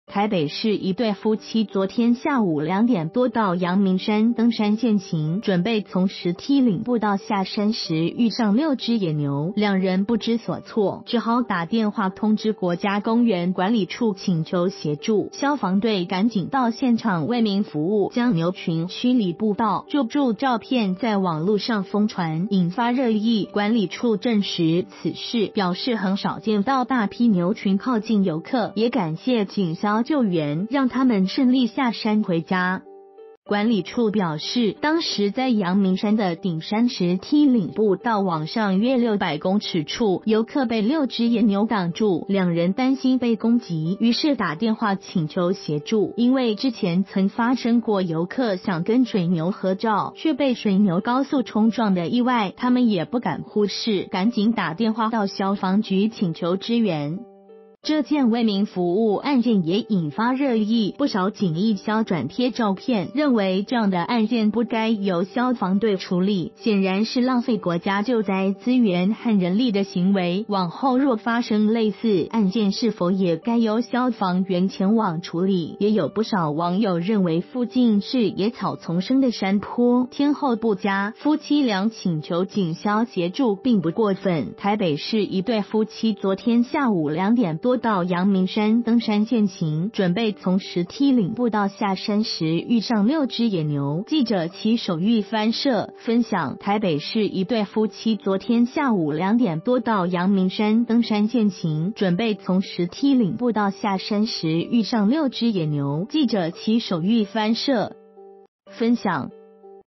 陽明山上兩夫妻被牛包圍 call消防隊上山趕牛
台北市一對夫妻昨天下午兩點多到陽明山登山健行，準備從石梯嶺步道下山時遇上6隻野牛，兩人不知所措，只好打電話通知國家公園管理處請求協助，消防隊趕緊到現場「為民服務」，將牛群驅離步道，救助照片在網路上瘋傳，引發熱議；管理處證實此事，表示很少見到大批牛群靠近遊客，也感謝警消救援，讓他們順利下山回家。